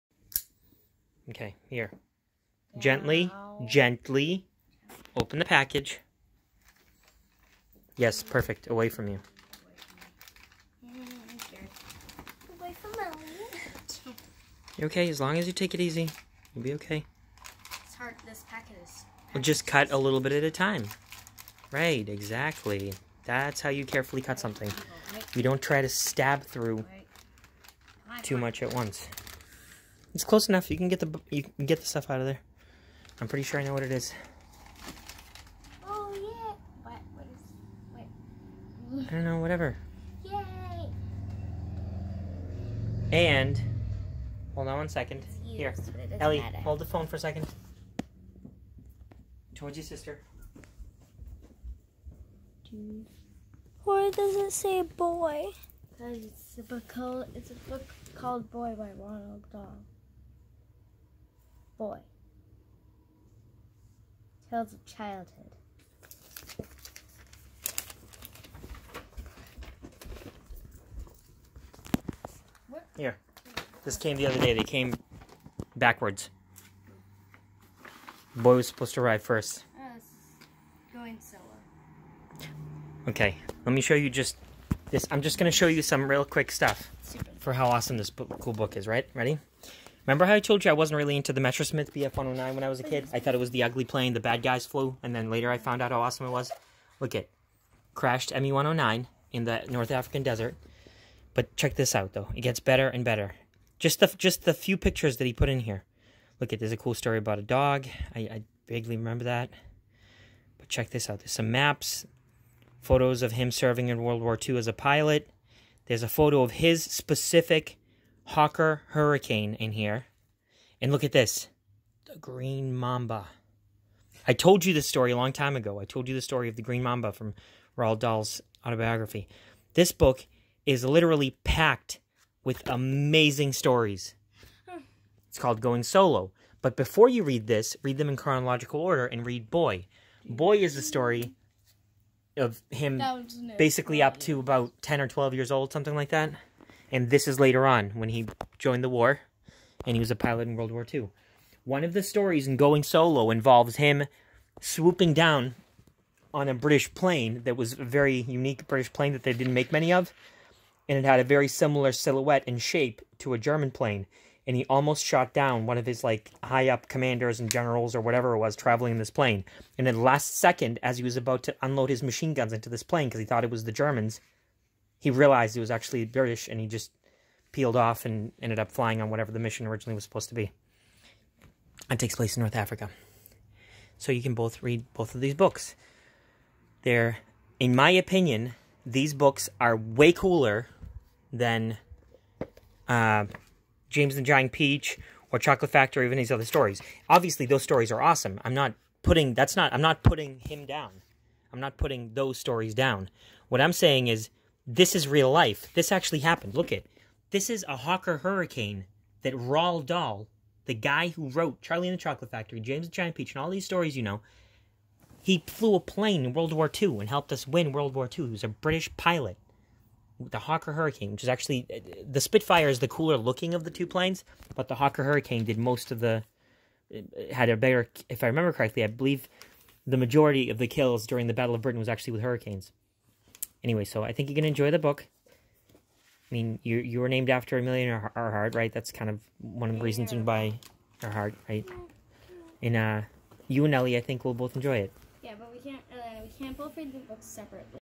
Roald Dahl's Autobiographies: eBay finds
I bought the books "Boy" & "Going Solo" for Amelia & Eliana